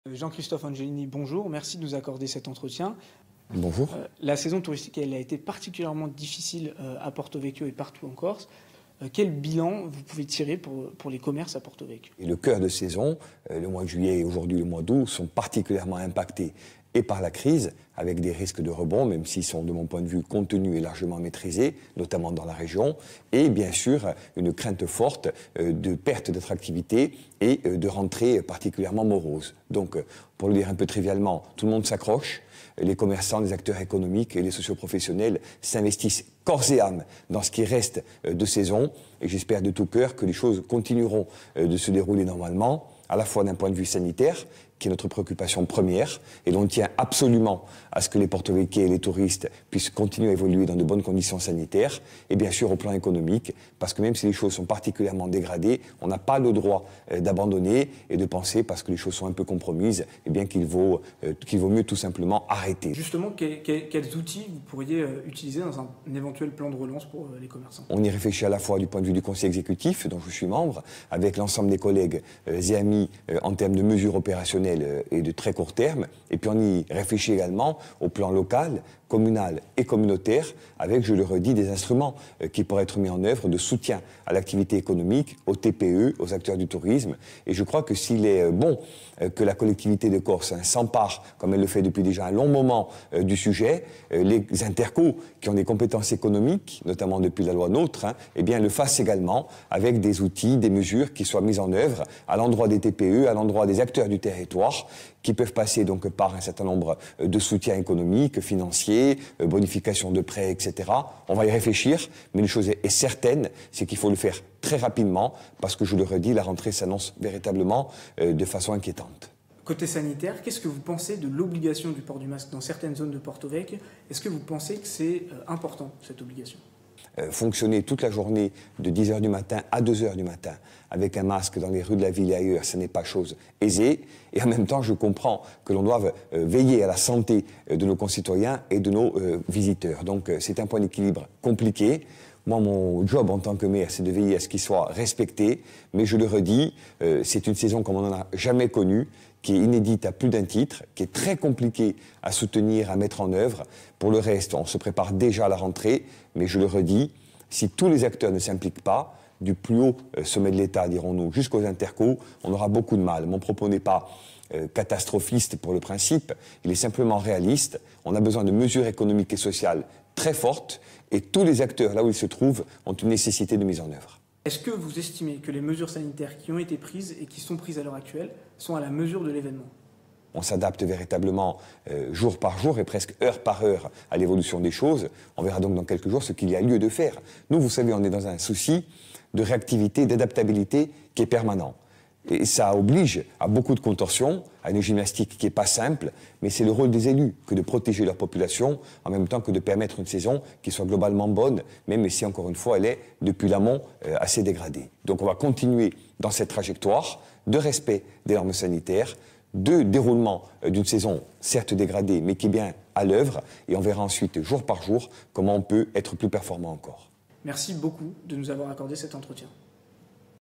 Tourisme et coronavirus : entretien avec Jean-Christophe Angelini
Cette année, à Porto-Vecchio, comme dans le reste de la région les professionnels du tourisme sont inquiets. Après un mauvais début de saison, ces deux dernières semaines semblaient marquer une amélioration. Mais le port du masque obligatoire semble freiner cette dynamique. Entretien avec Jean-Christophe Angelini, maire de Porto-Vecchio.

Intervenants - Jean-Christophe Angelini, maire de Porto-Vecchio.
Equipe - Clément Tronchon, Tiphaine Niederlaender, Frédéric Guichard.